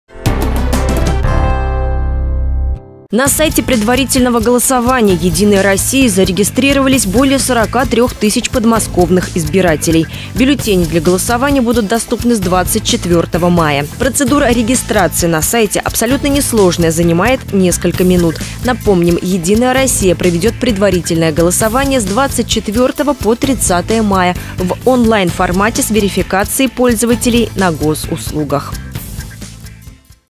Предварительное голосование
На сайте предварительного голосования «Единой России» зарегистрировались более 43 тысяч подмосковных избирателей . Бюллетени для голосования будут доступны с 24 мая.